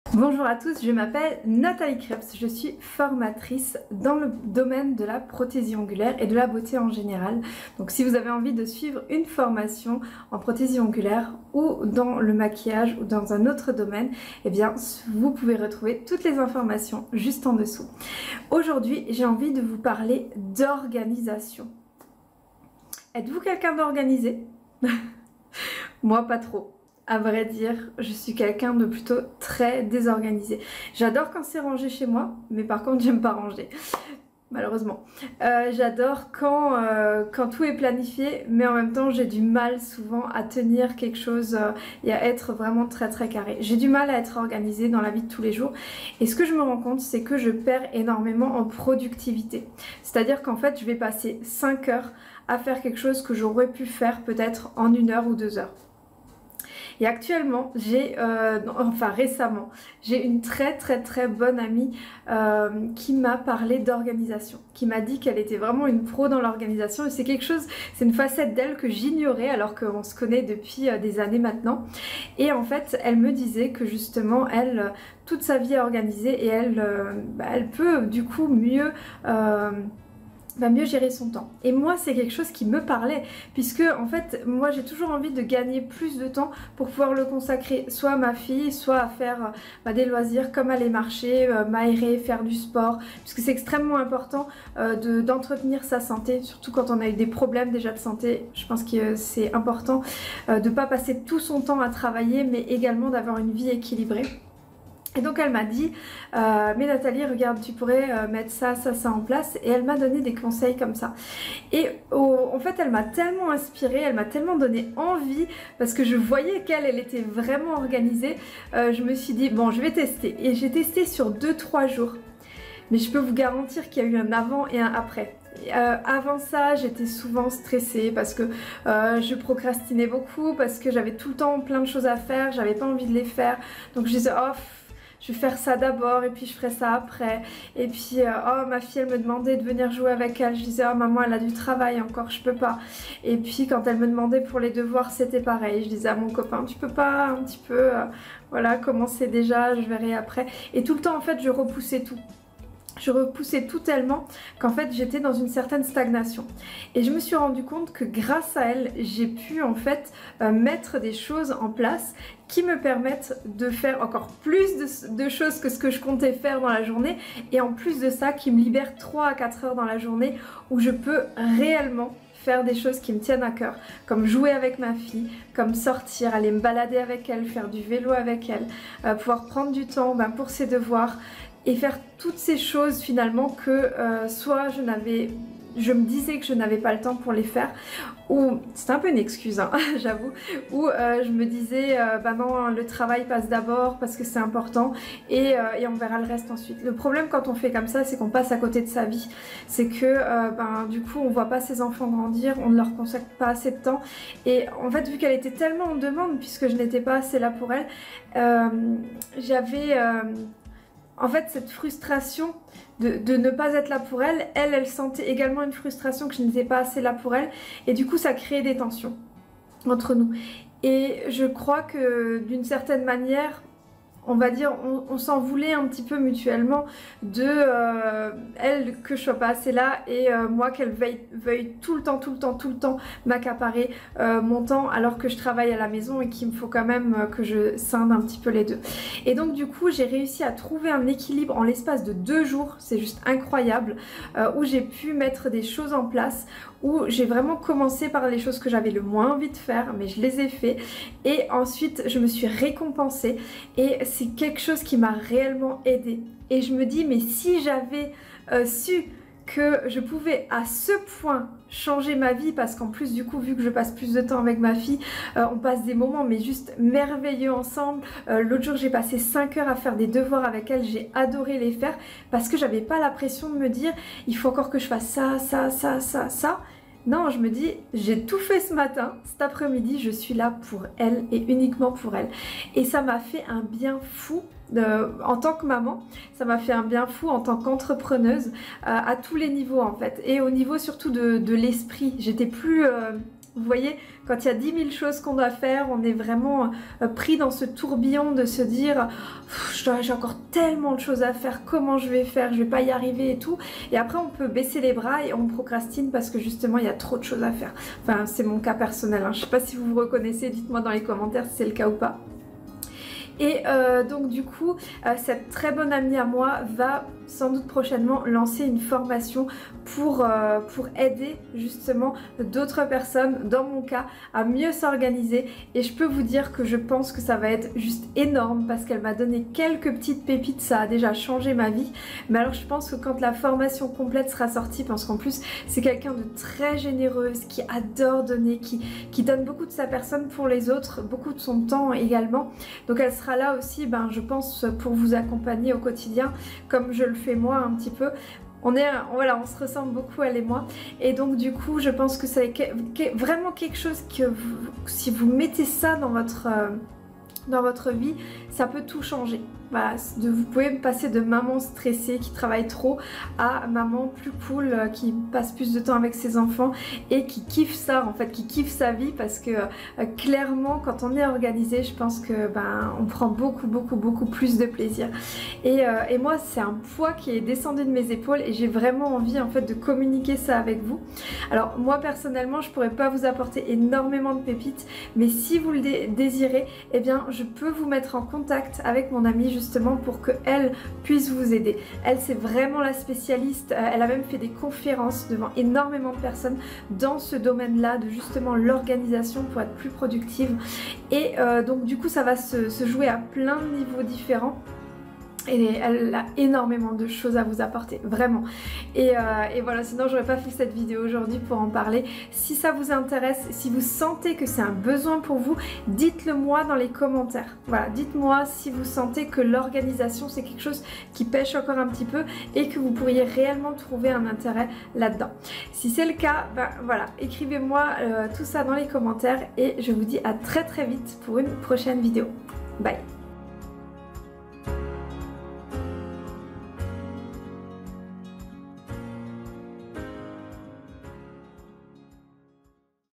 Comment l'organisation peut changer ta vie I Yournails International
Dans cette vidéo je vais vous parler de l'organisation. Si vous êtes comme moi et que vous avez des milliers de choses à faire au quotidien, nous vivons alors les mêmes difficultés. Vingt quatre heures ne sont parfois pas suffisantes pour pouvoir profiter pleinement des journées. Si vous avez la sensation que vous manquez de STRATÉGIE pour mieux gérer votre temps, alors vous êtes sur la bonne vidéo ;)

DES QUESTIONS?